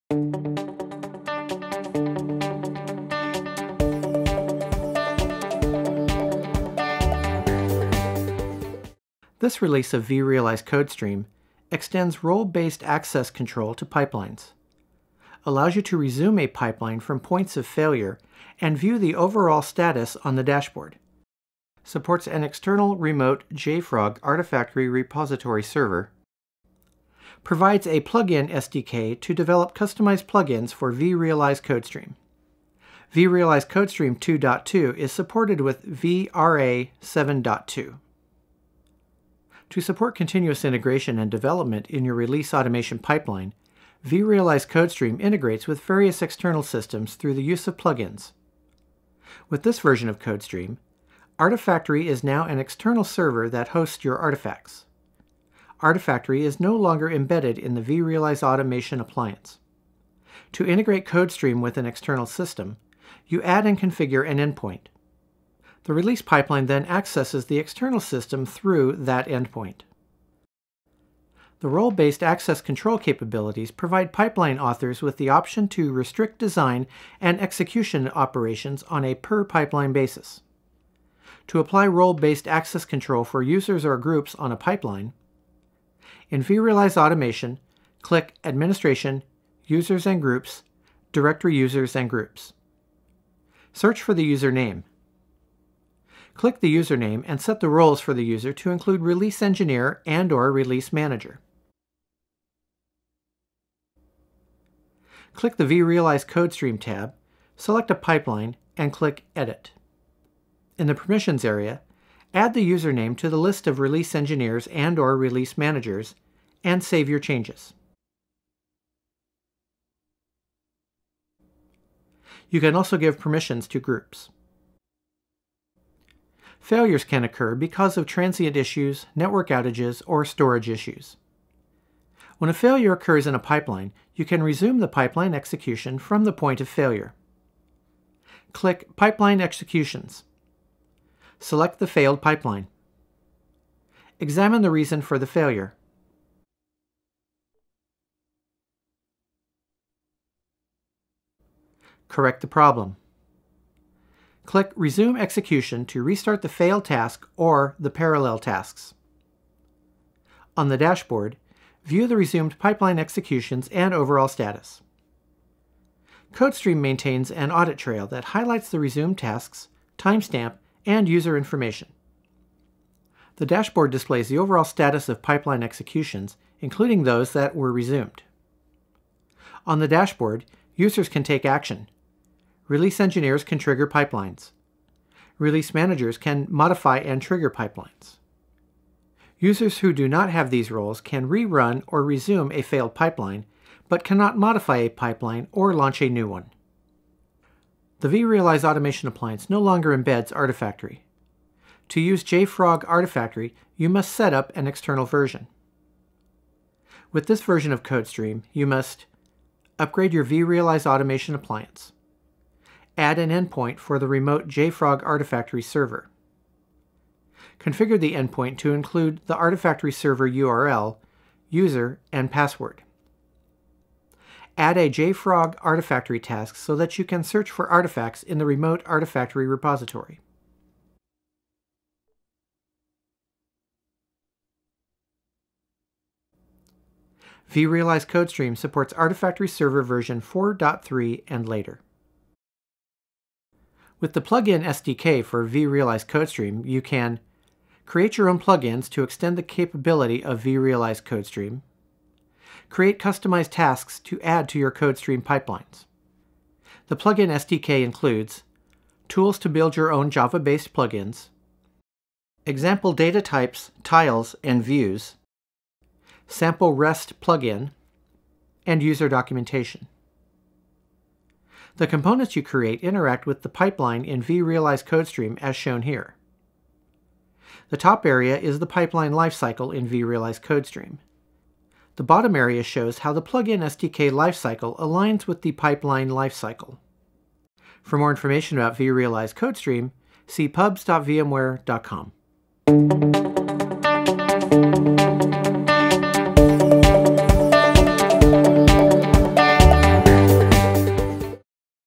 vRealize Code Stream 2 2 Overview and What's New
This video give an overview of what's new in vRealize Code Stream 2.2